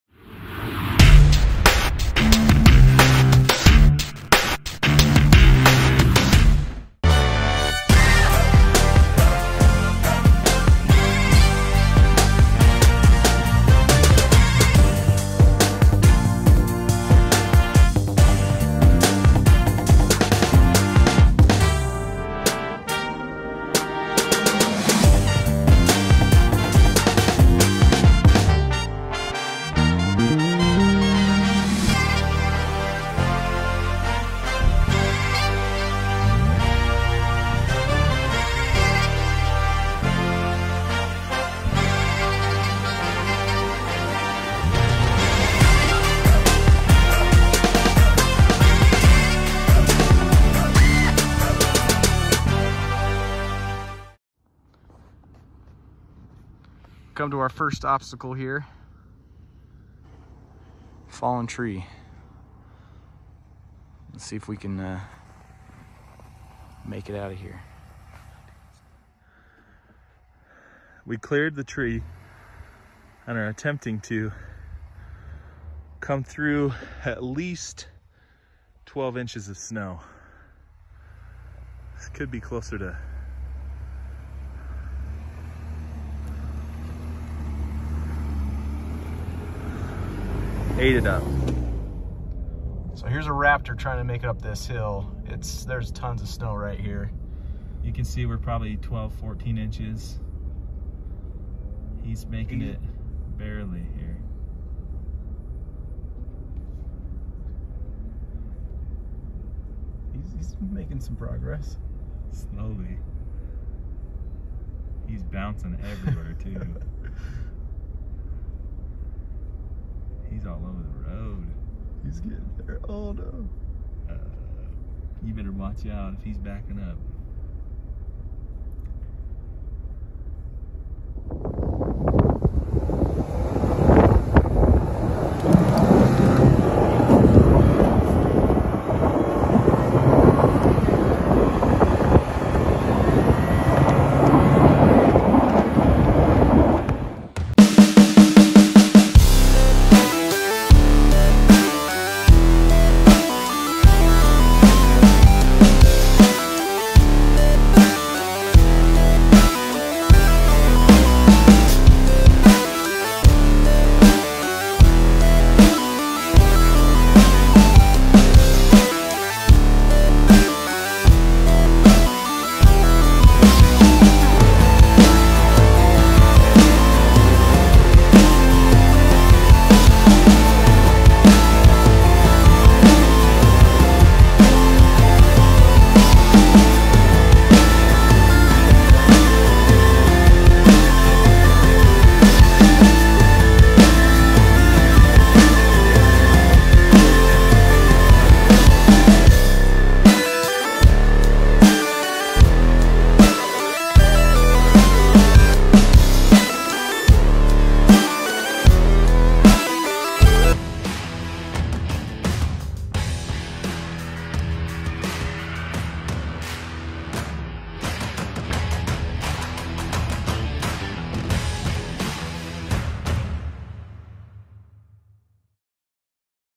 2021 Ford BRONCO Wildtrak Vs. The Snow!
We take the new 2021 Ford Bronco Wildtrak with the sasquatch package on a snowy trail to see how it handles. We did not air down and it handled very well. We came across a raptor trying to blaze the same trial but, he got stuck and we continued forward all the way to the peak.